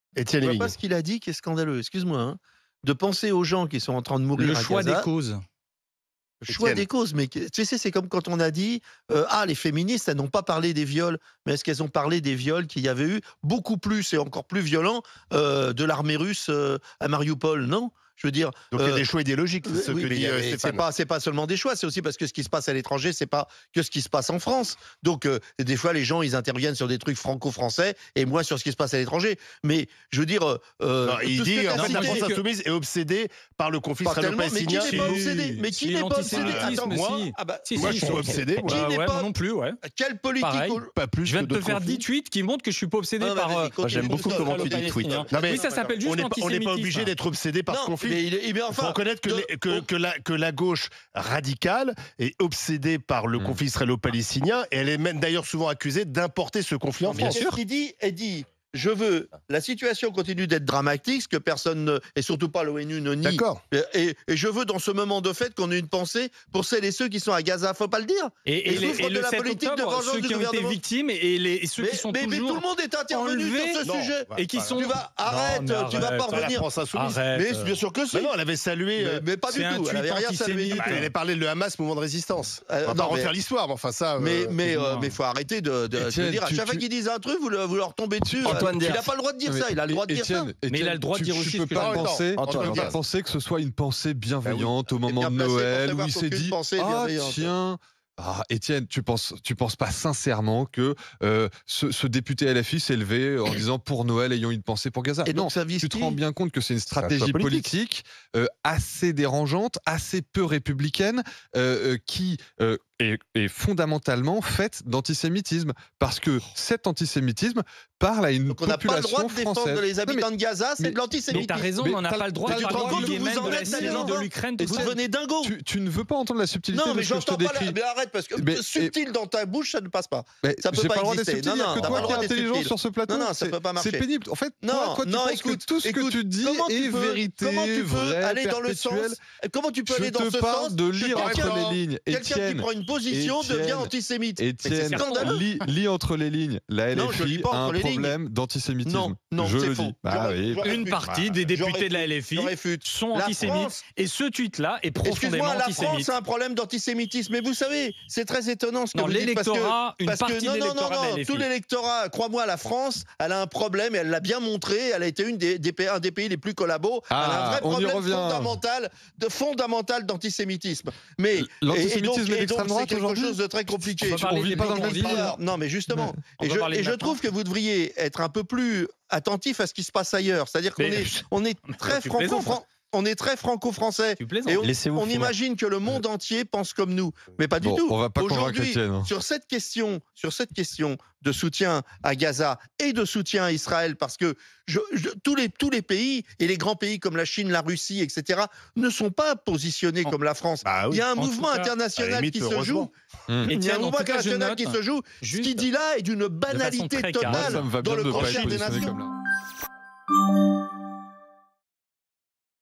Défendre GAZA c'est antisémite ?
Les commentaires du député LFI Antoine Leaument, exprimant sa sensibilité envers la souffrance des Palestiniens et sa volonté de dédier Noël à la population de Gaza, ont suscité un scandale en France.

Ses propos ont été qualifiés d'"antisémites" lors d’une émission sur RMC.

Etienne Liebig, présent lors de l'émission, a réagi à cette qualification, tandis que Antoine Diers a affirmé que la LFI avait déjà un problème d'"antisémitisme".

"Jamais je n’aurais pensé que pour avoir dit - très simplement - que dans un moment de fête, je voulais qu’on ait une pensée pour ceux qui souffrent, on me repeindrait en antisémite contre-républicain. Se rendent-ils compte d'où ils en sont ?", a répondu Antoine Léaument, dans une publication sur son compte X.